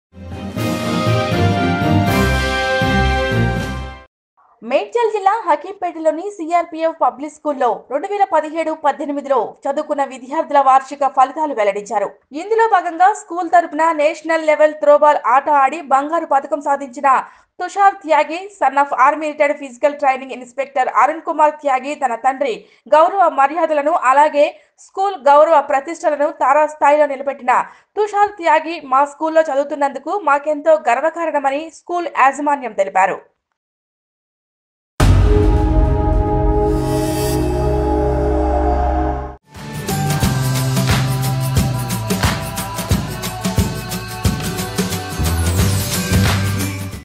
బంగారు పథకం సాధించిన విద్యార్థి తుషార్ త్యాగి || Nethra News
|| Nethra News || హకీంపేట్ లోని CRPF పబ్లిక్ స్కూల్ ||